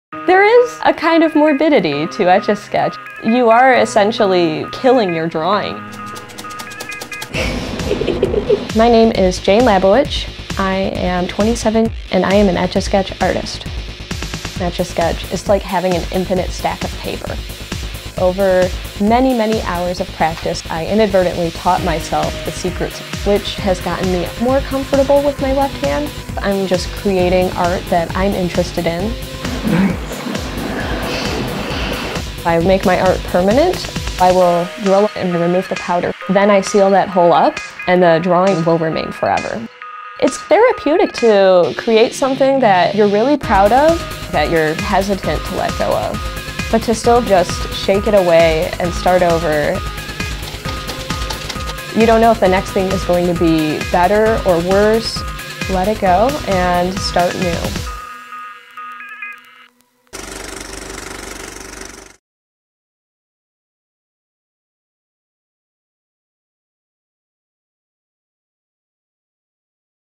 This Princess Creates Masterpieces On Etch A Sketch
Jane Labowitch otherwise known as Princess Etch is a professional Etch a Sketch artist. She uses the toy as a canvas for elaborately detailed works of art that can sell for up to $1500. The Chicago-based artist now takes commissions from companies like Cartoon Network, Disney and CNN. No matter how big her art gets she says there’s just something therapeutic to, “...shake it away and start over.”

60 Second Docs